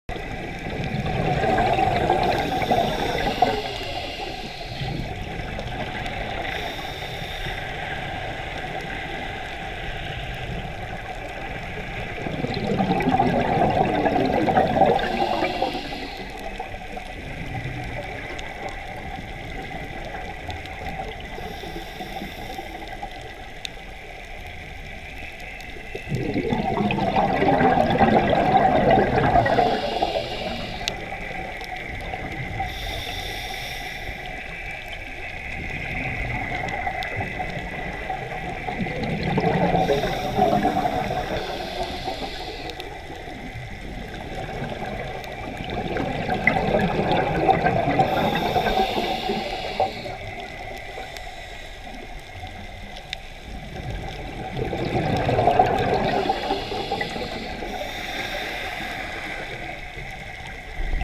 Mantas and more mantas
Mantas spotted! Maldives scuba diving adventure.